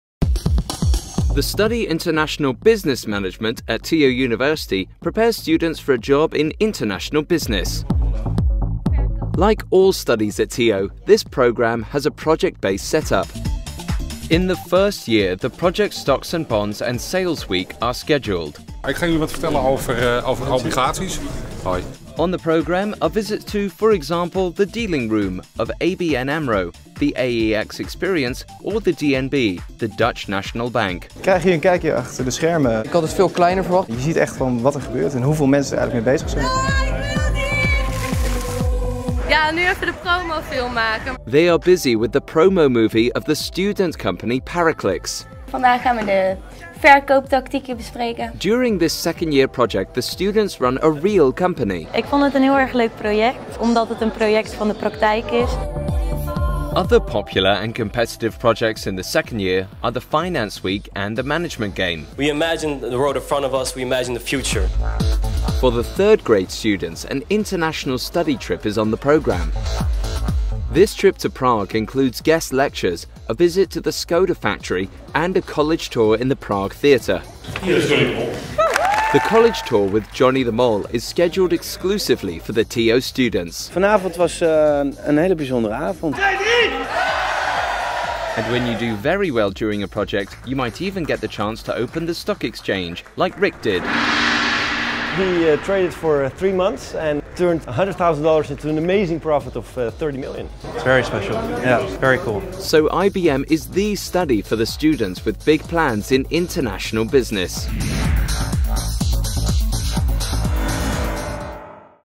Business Management
Wil jij de beste deals sluiten en internationaal contacten leggen? Misschien is de hbo-opleiding International Business Management bij Hogeschool Tio dan wel iets voor jou! Bekijk de video om meer te weten te komen over de projecten die je gaat volgen. De praktijkervaring die je opdoet tijdens de projecten gaat jou zeker helpen bij je carrière! Kijk voor meer info op